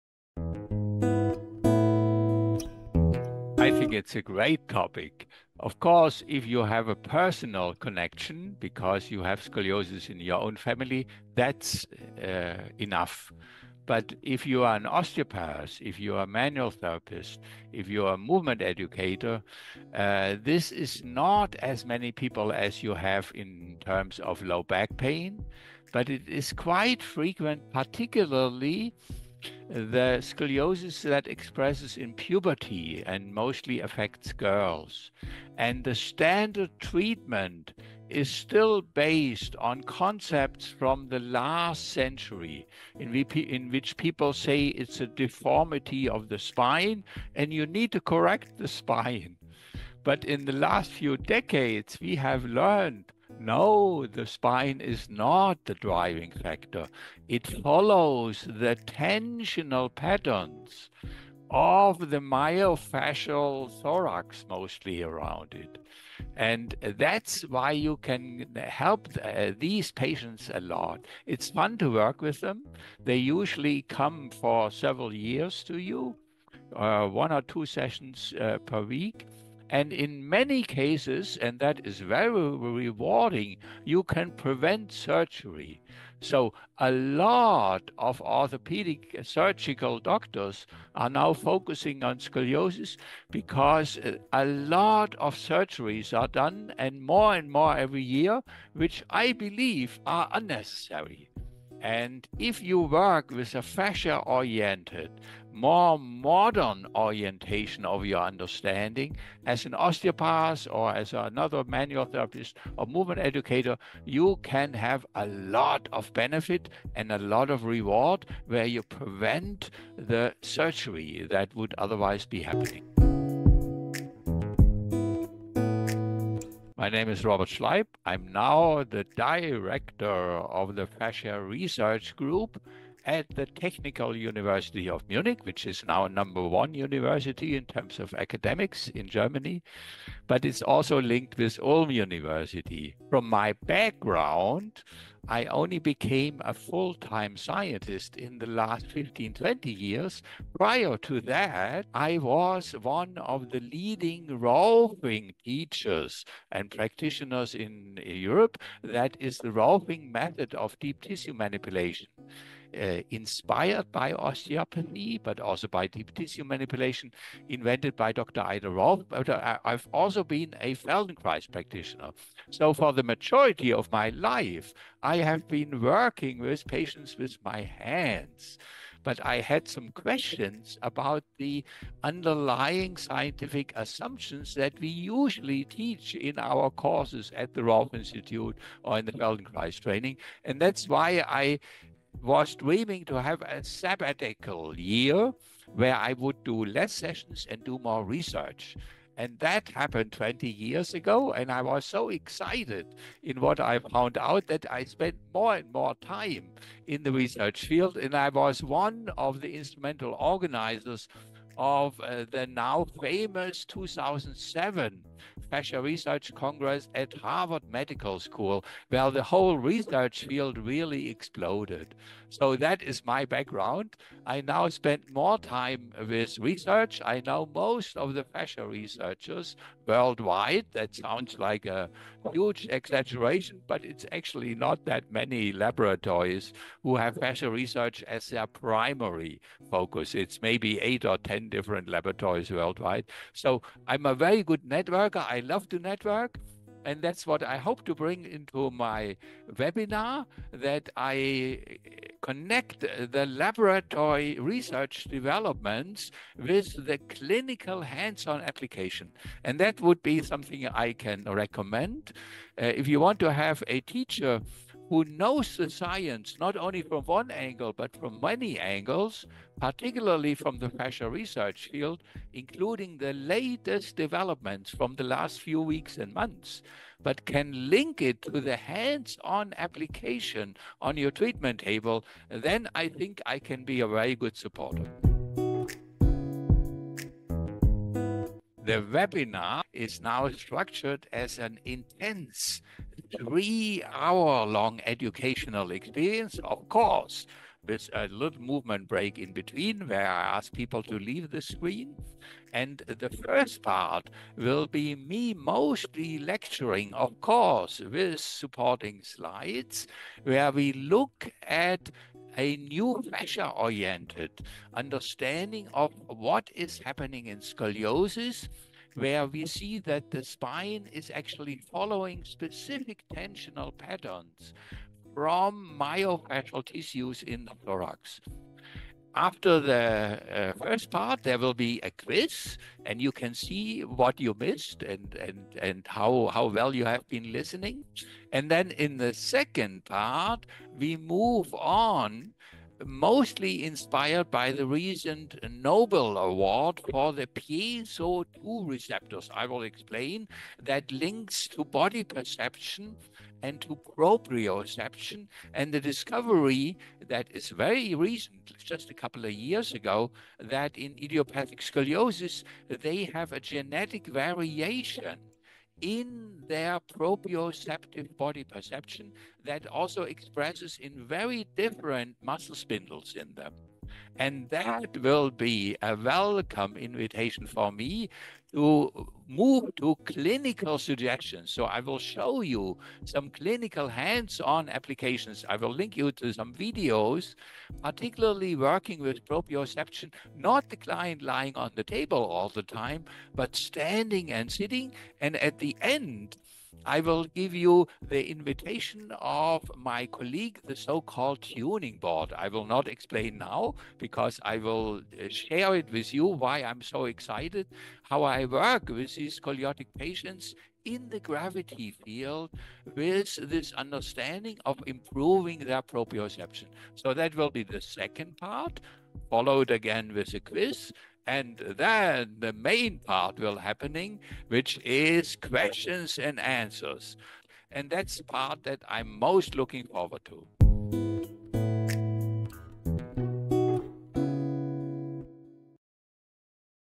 Scoliose et Fascias : une nouvelle lecture par le Pr Robert Schleip
Cette webconférence animée par le Pr Robert Schleip (Fascia Research Group-Université de Munich) propose une approche inédite de la scoliose idiopathique de l’adolescent (SIA) centrée sur le fascia, la neurobiologie et la physiologie circadienne.

📣 Webconférence **Scoliose & Fascias : une nouvelle lecture - Pr Robert Schleip**
🔆 Traduction simultanée ANG-FR par Marjolaine Dey, ostéopathe DO
💥 Présentation par Nicolas Reinié, ostéopathe DO
📆 Sur Zoom Le Jeudi 19 Juin 2025 de 19h à 22h - H de Paris +  Replay

✅️ Le riche programme intègre notamment :
-L’effet des tensions myofasciales (médiastin, psoas, diaphragme) sur la courbure spinale.
-La proprioception altérée liée au gène PIEZO2 et des exercices pour la réhabiliter.
-L'enrichissement de l'examen clinique pour tous les acteurs de la pédiatrie
-L’influence des rythmes circadiens sur le remodelage du collagène et l’équilibre fascial.
-Des techniques manuelles ciblées pour travailler sur les tensions fasciales.
-La « tuning board » : un outil innovant pour améliorer l’intégration neuromyofasciale.

👉 Restez jusqu’à la fin pour le Q/R en direct avec le Dr Schleip !

📌 Objectifs :
-Comprendre le rôle central du fascia dans la SIA.
-Identifier l’influence des schémas de mobilité et de la génétique.
-Intégrer la biologie circadienne pour optimiser vos interventions.
-Mettre en place des exercices psycho‐proprioceptifs adaptés à l’adolescent.

🙋‍♂️🙋‍♀️ À qui s’adresse cette conférence ?
Ostéopathes, thérapeutes manuels, kinésithérapeutes ou toute personne souhaitant enrichir sa pratique clinique par une approche fasciale et biopsychosociale de la scoliose.

💡 Toutes les formations dans le champ fascial